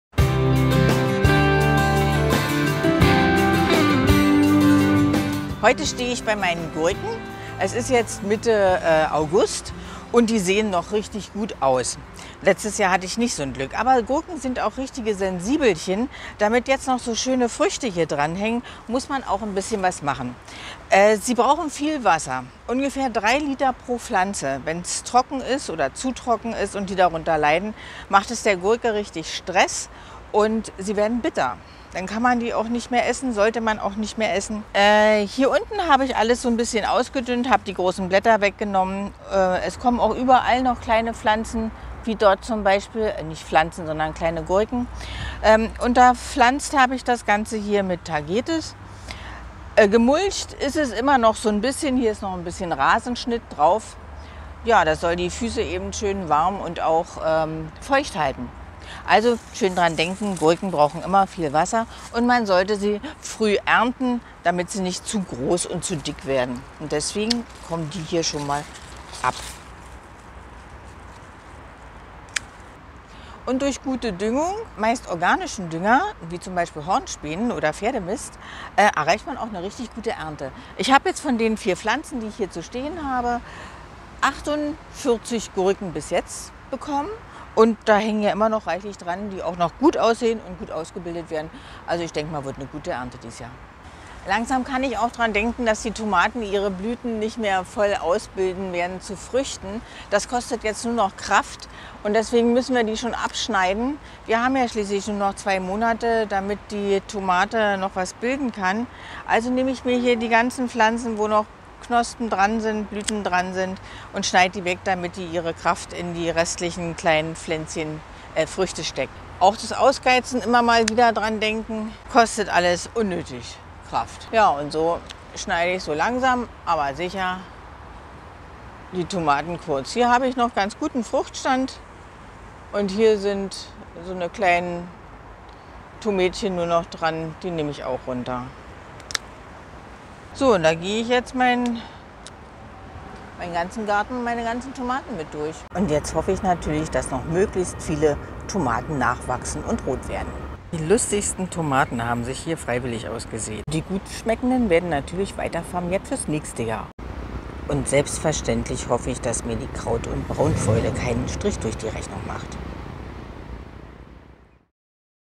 🥒 GURKEN 🥒 und 🍅 TOMATEN 🍅 im Spätsommer | Wie bekomme ich eine reiche Ernte👩🏻‍🌾?
Wie baue ich Gurken am besten an und wie geben sie besonders viele Früchte?

Jetzt ist die Zeit der Belohnung für die Mühe der letzten Wochen. Die Ernte wird eingefahren. Dieses Jahr haben bei mir die Gurken🥒 und Tomaten 🍅 besonders viele Früchte.
Was wurde eigentlich aus meinen vielen wilden Tomaten 🍅?